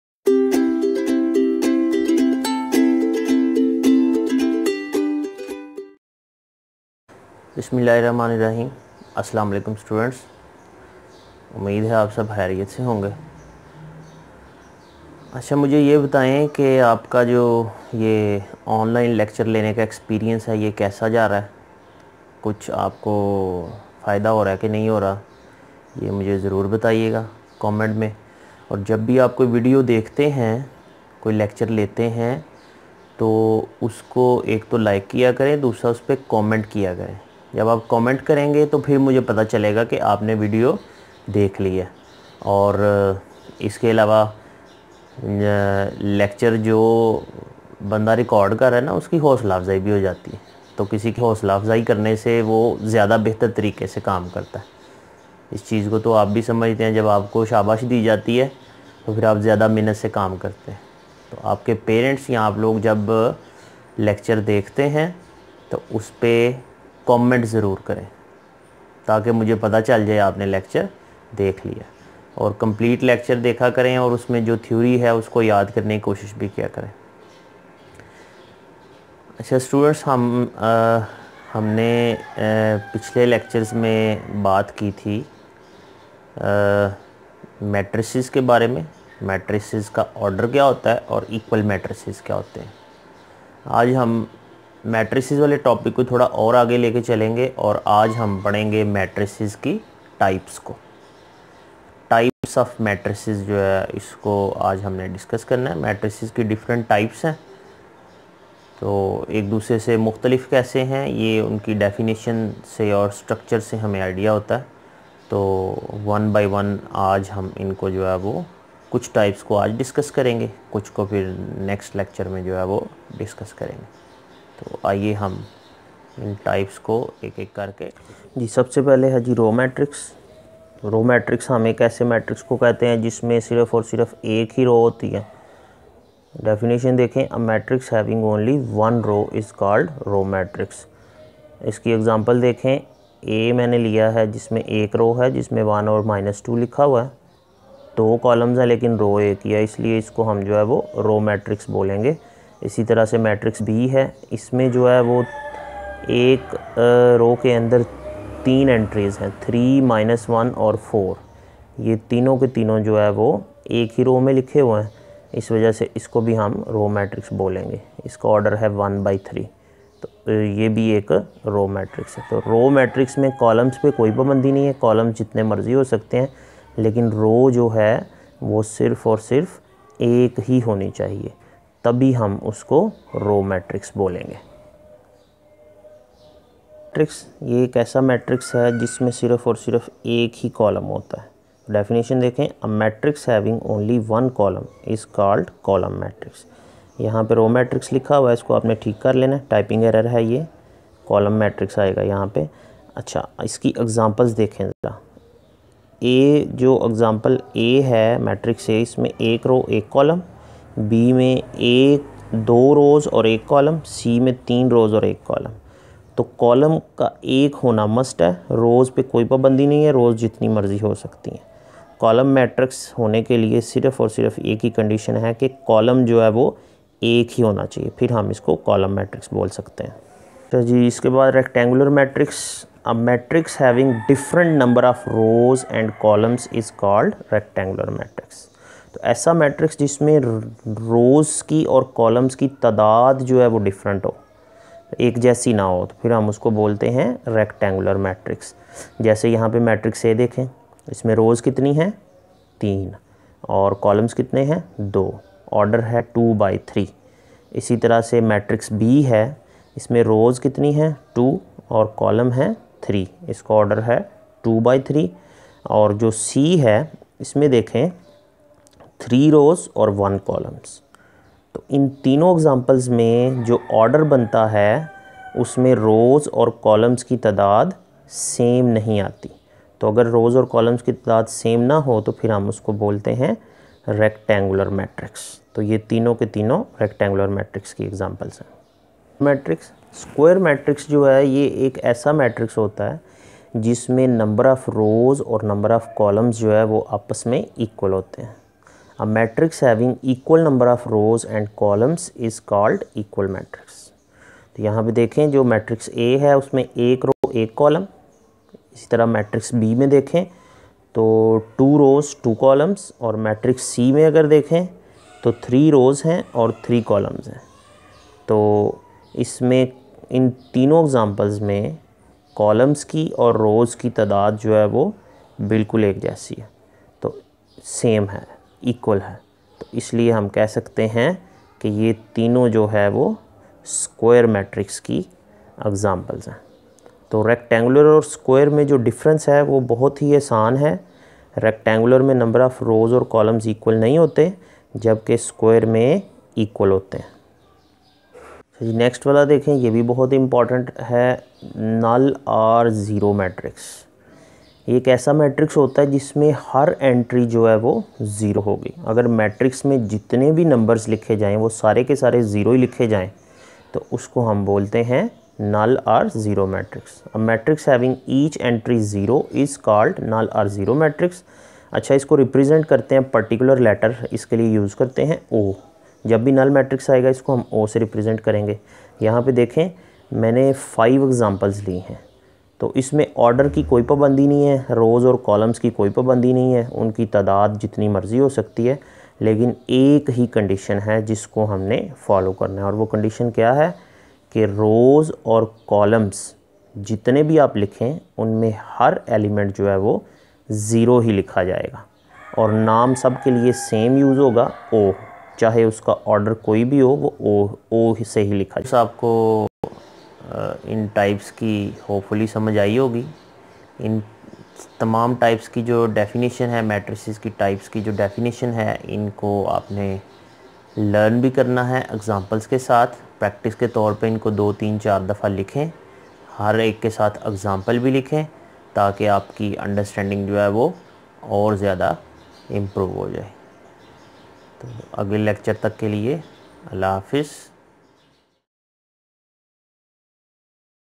9th Maths Ch#1 Lecture#4 Types of matrices
In this video, I have explained types of matrices with examples.